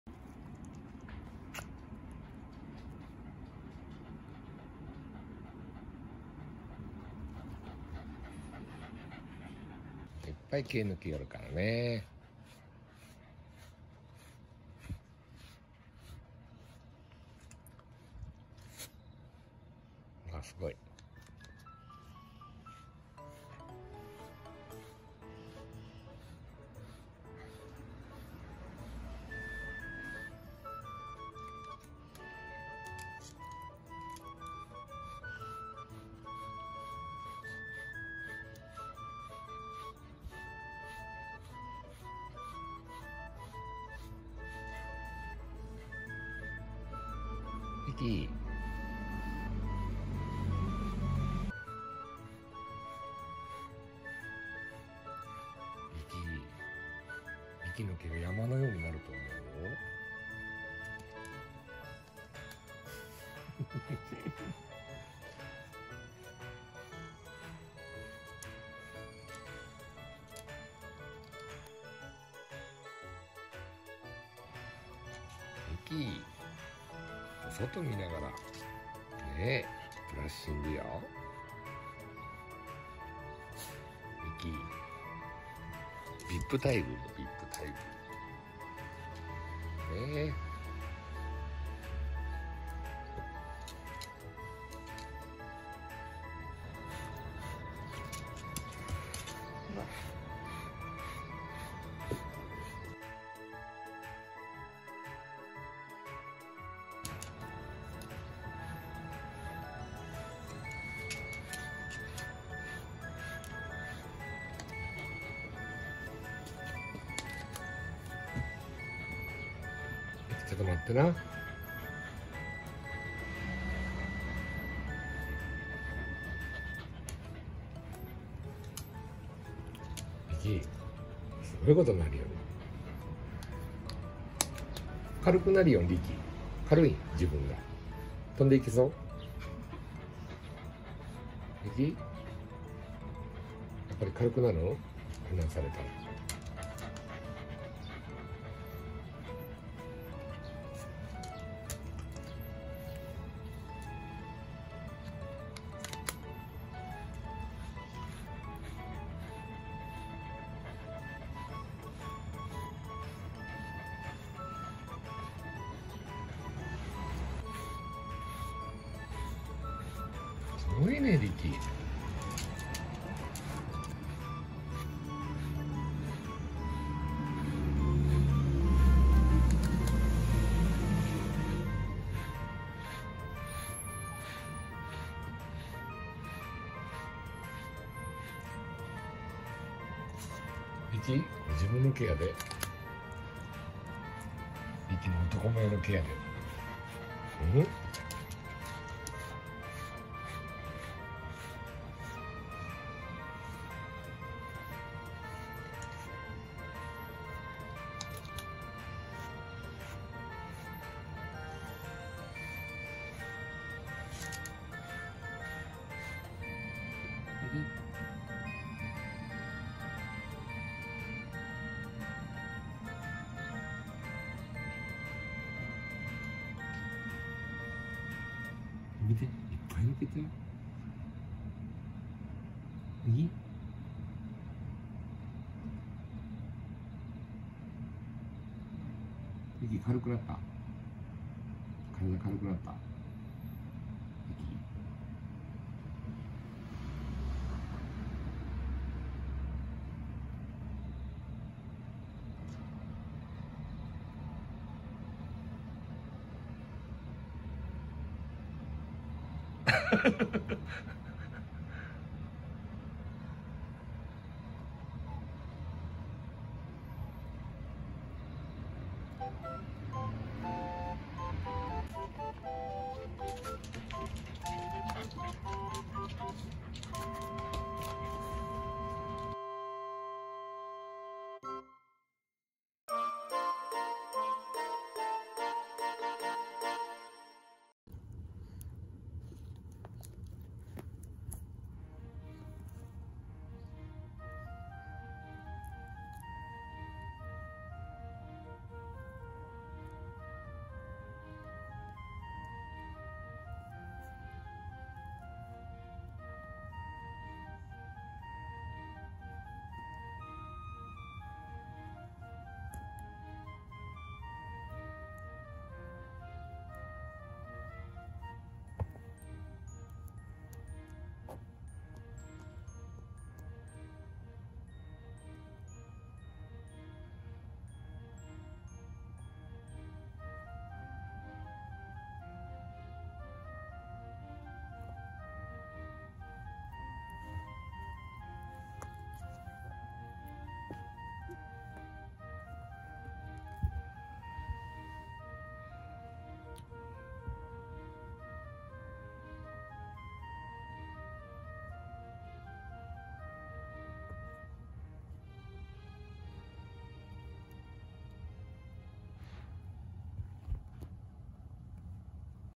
短時間でみるみるうちに痩せていく柴犬と柴犬のモコ毛に顔を埋める子猫♪
ブラッシングで、小型犬が出来そうなレベルです！！

窓際で、優雅にVIP待遇なリキ♥
他の皆は毛が舞ってしまうので、お部屋で待機中です(#^^#)

みるみるうちに、リキが痩せていく？？（痩せてません(笑)）
何だか、前よりスマートになったような気がします(*´艸`)

ブラッシングが終わったら、お部屋を開放♪

一番乗りは子猫リク♪
来るやいなや、リキのモフ毛にズボっ！！！

大好きなリキの匂いで思わずしちゃったのかな？(*´艸`)

こちらにも、遊びに来てくださると嬉しいです😸🐶

リタ　キジトラ猫                女の子
リク　茶トラ子猫                男の子
リノ　白MIX子猫　　　　　　　　女の子

Kitten                 Riku (male)
Kitten                 Rino(female)

BGM
甘茶の音楽工房